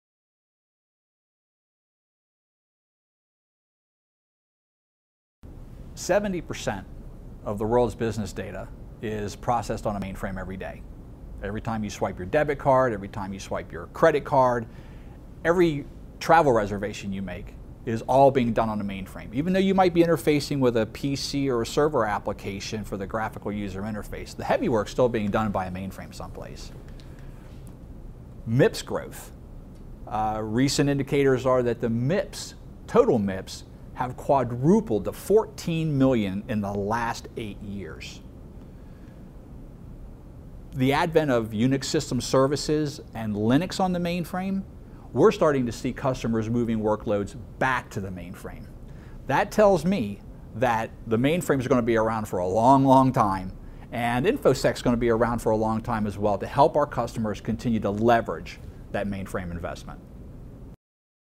What is the future of the mainframe computing platform?
Insights from InfoSec CEO Bob Fake.